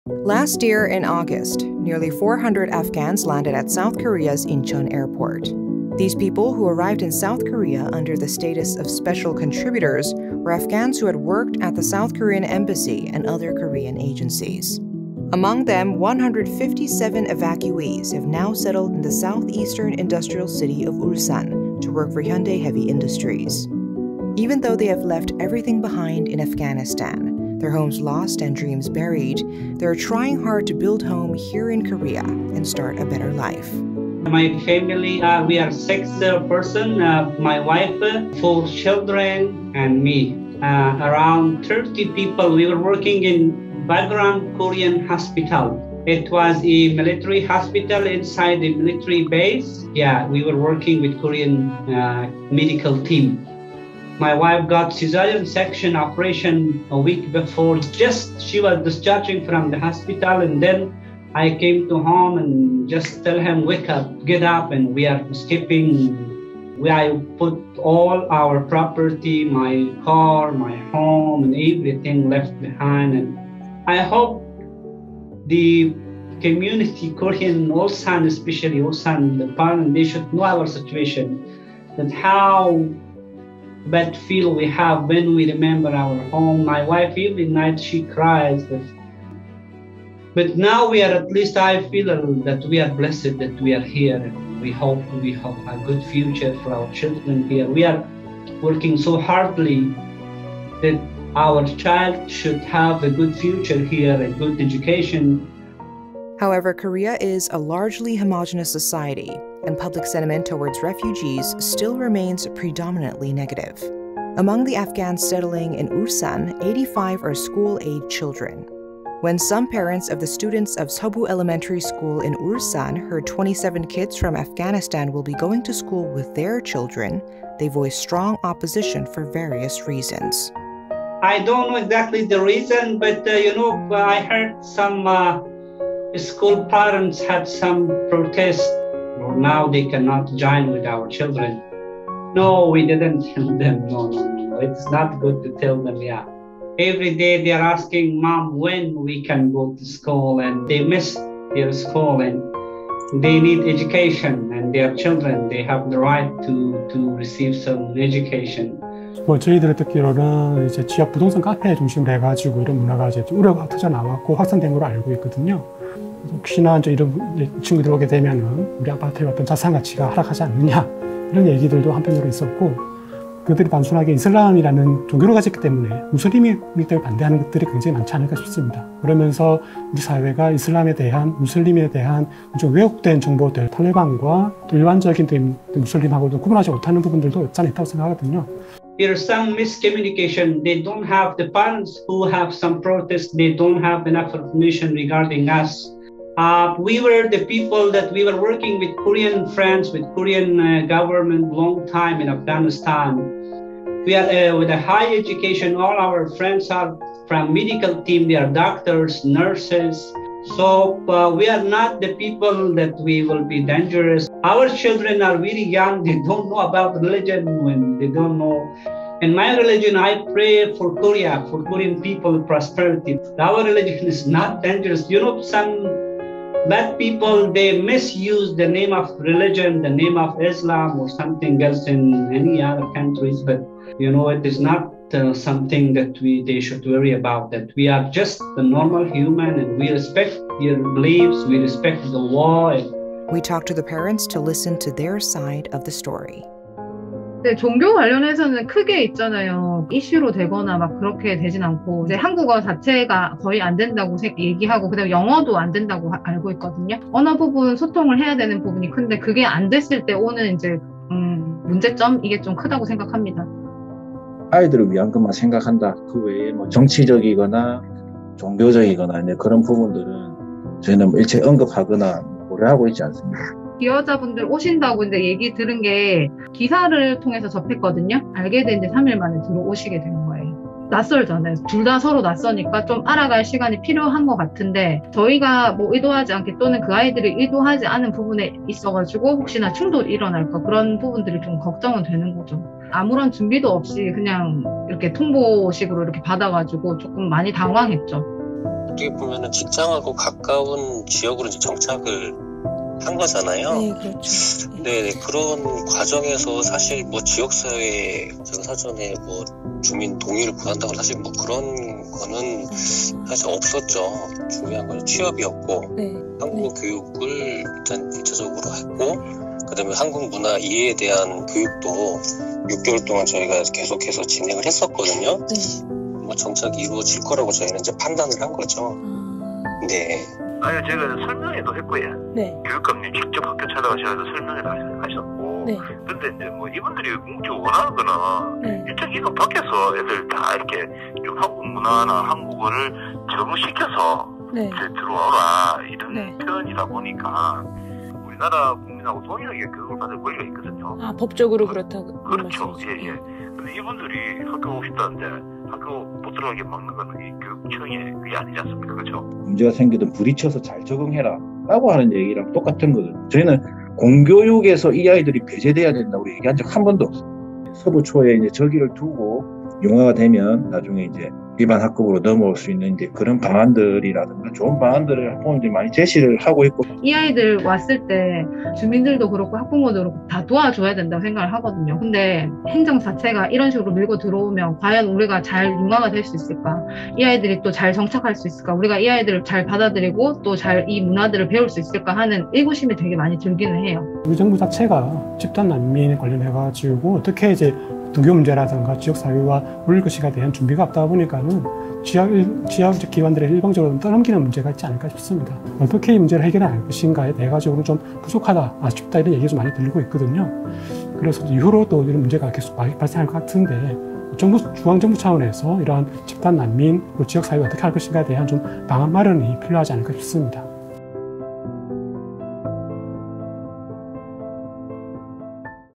Afghan refugee children unwelcome at Korean school
While students of Seoboo elementary school in Ulsan headed to classes on the first day of the spring semester on March 2, 9-year-old Zainab from Afghanistan, who moved to the southeastern port city last month, had to stay at home.

She asked her parents when she would be able to join her peers at school, but neither of them gave her a clear answer, which left the girl more puzzled.

Her father, Gul Agha (as he prefers to identify himself), could not tell his daughter the truth: due to fierce protests from their Korean neighbors, it remains unclear when Zainab would be able to attend the public school.

Article by Lee Hyo-jin
Video by Lee Min-young, Kim Kang-min